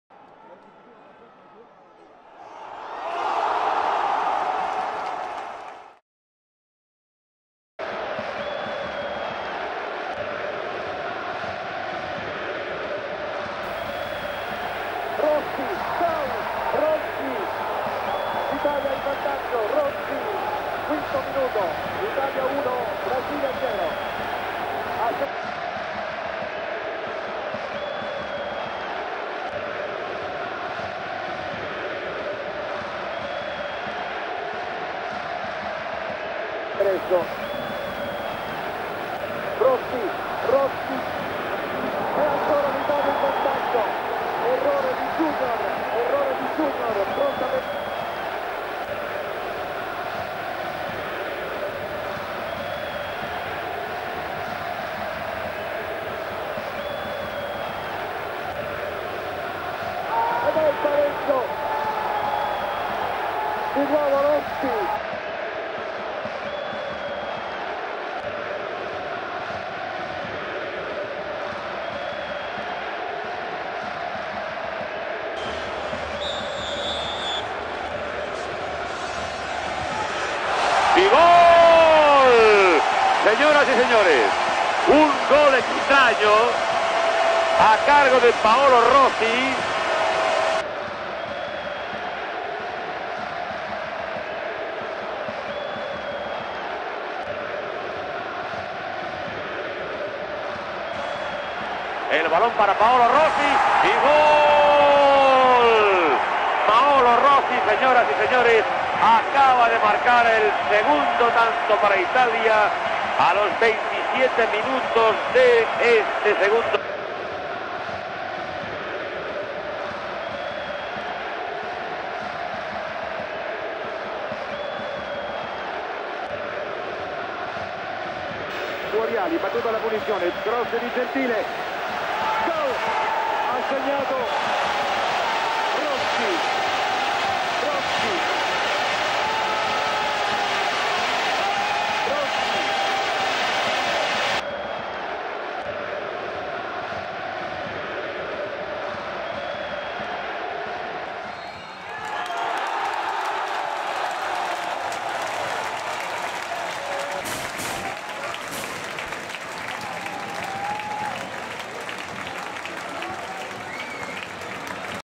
Paolo Rossi Golden Boot Winner at the 1982 World Cup
Paolo Rossi led his national side to the title during the 1982 World Cup. He scored six goals in winning the Golden Boot. He is currently Italy's joint-top scorer in World Cup history, with nine goals in total.